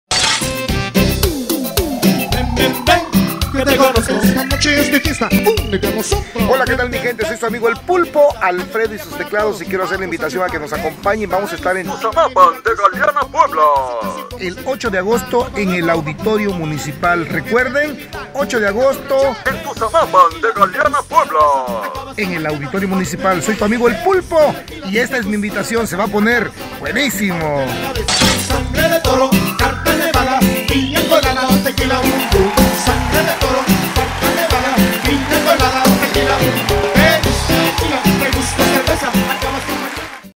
Alfredo y sus Teclados "El Pulpo" invita al baile de Tuzamapan de Galeana, Puebla
Contrataciones a los teléfonos:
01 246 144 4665//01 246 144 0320//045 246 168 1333
*El Pulpo invita al bailazo de Tuzamanan de Galeana, Puebla, este 08 de agosto